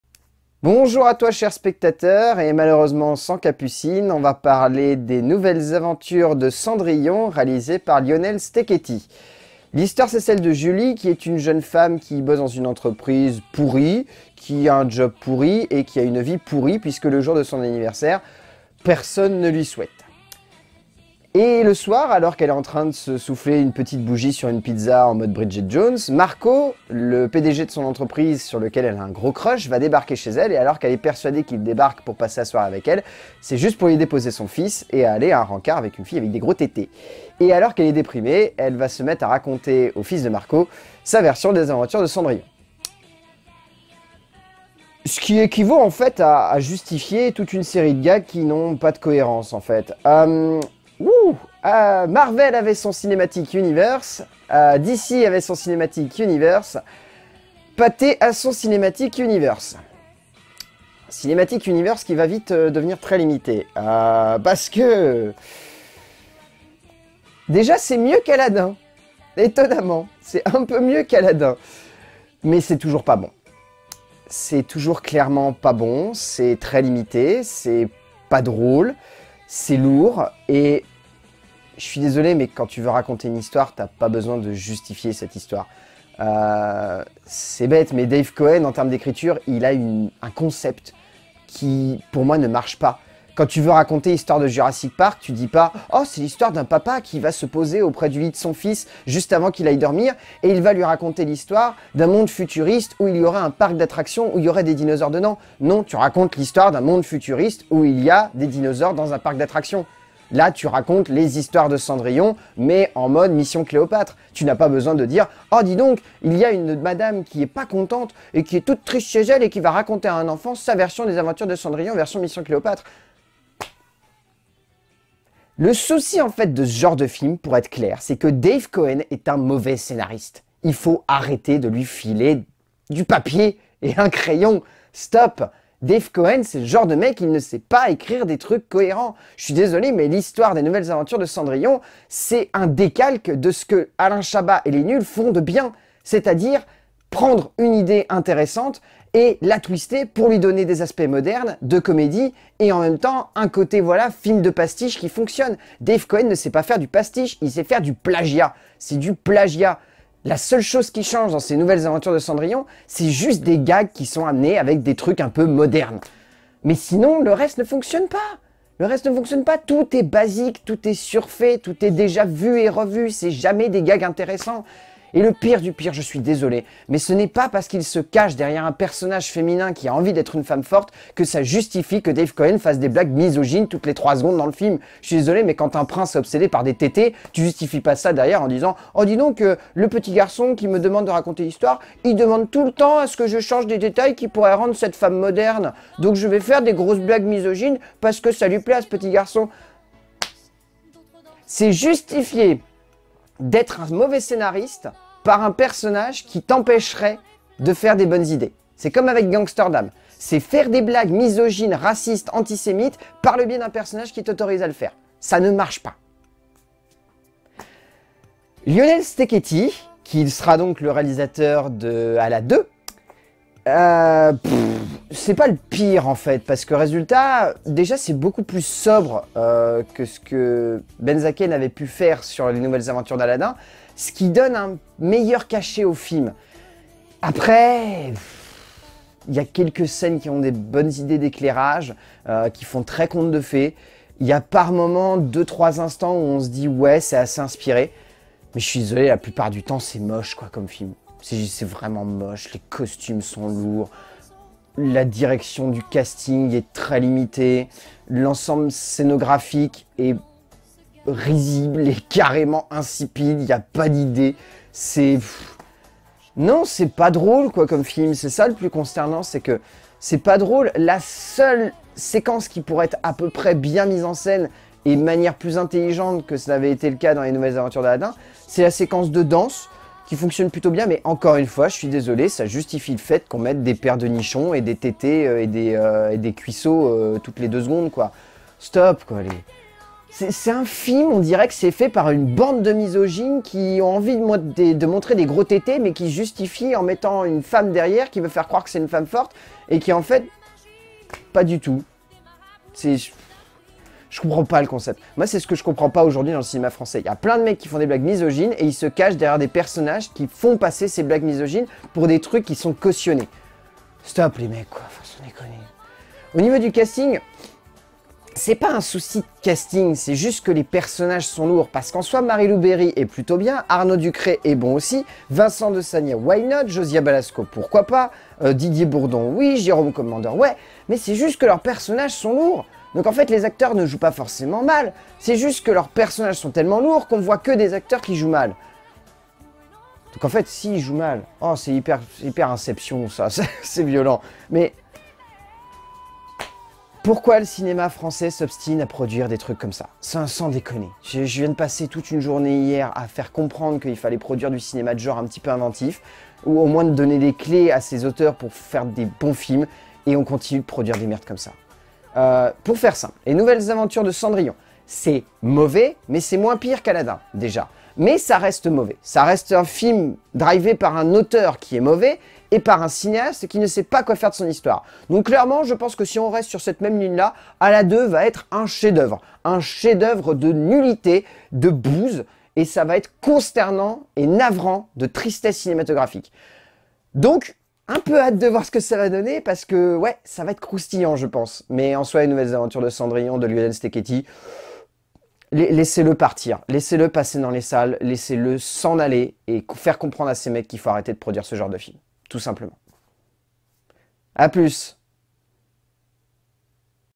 LCDC - Les nouvelles aventures de Cendrillon
Vlog critique sur la sortie d'un objet cinématographique inutile, vain, crétin, facile, cliché, bref, un film qui n'aurait jamais du voir le jour tout simplement, "Les nouvelles aventures de Cendrillon"